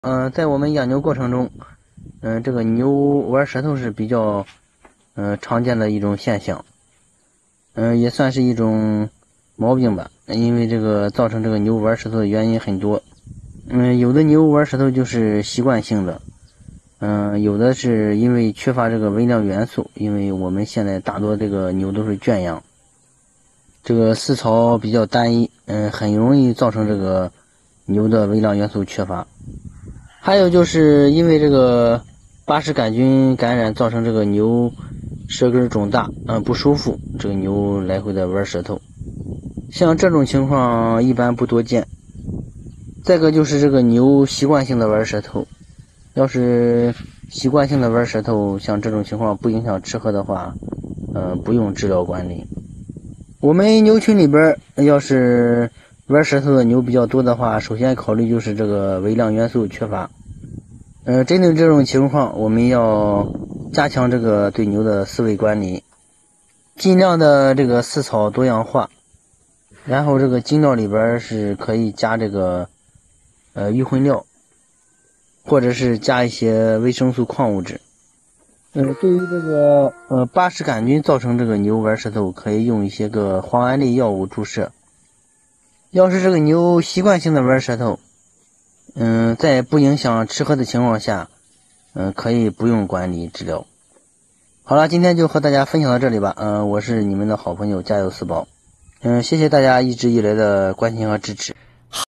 养牛过程中遇到牛玩舌头该怎么办
养牛过程中遇到牛玩舌头该怎么办,本视频由快乐养牛人家有四宝原创提供,3803次播放,好看视频是由百度团队打造的集内涵和颜值于一身的专业短视频聚合平台